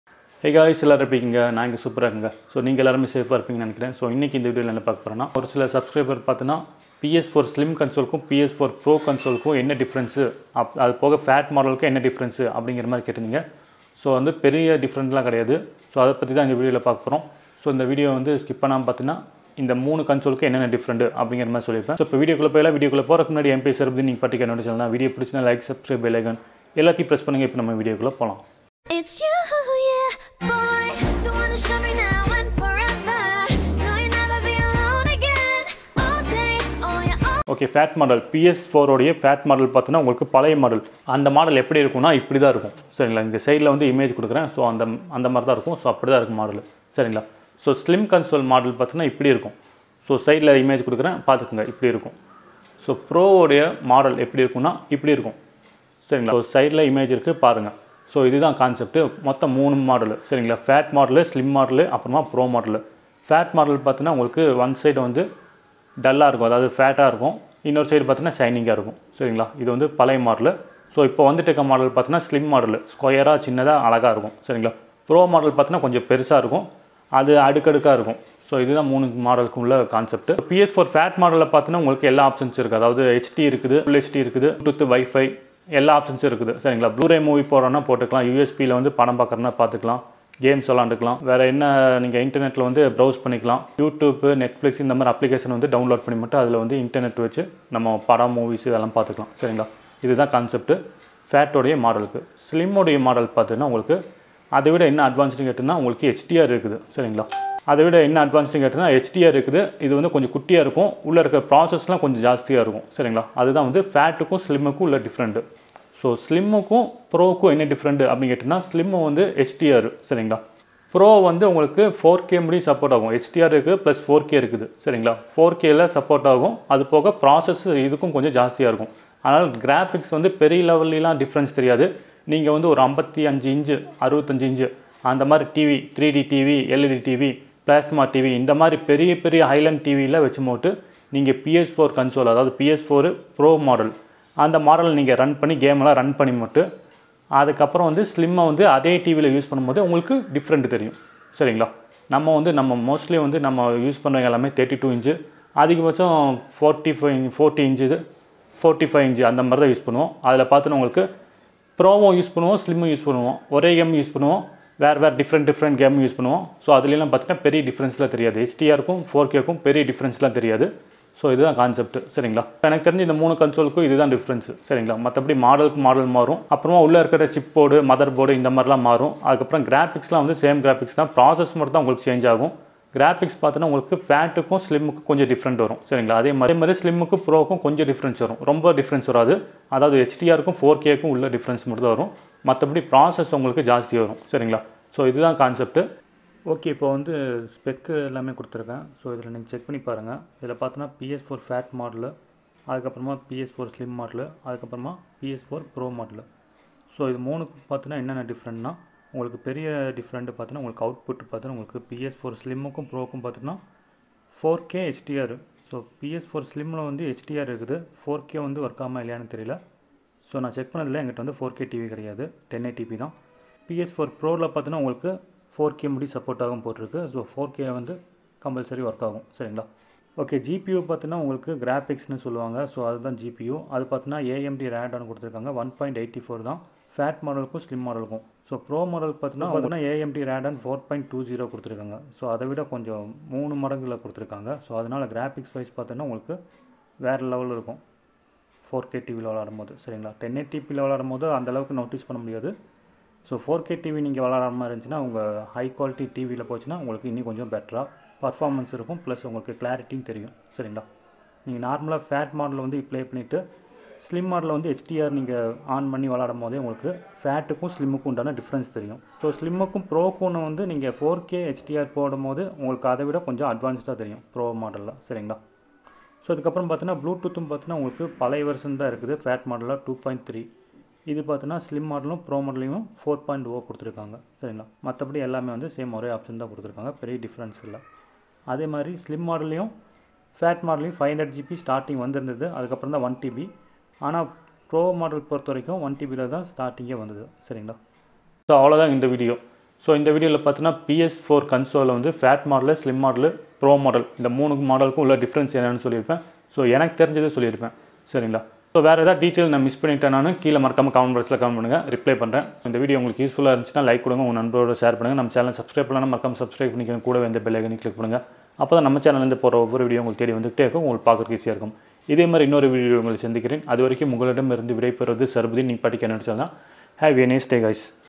PS4 FAT | SLIM | PRO | DIFFERENTS AND REVIEWS TAMIL தமிழ் 2021
PS4 FAT,SLIM,PRO INTHA 3 MODELS KU ULLA DIFFRENTS INTHA VIDEO LA SOLIRUPEN SKIP PANAMA VIDEO PAARUNGA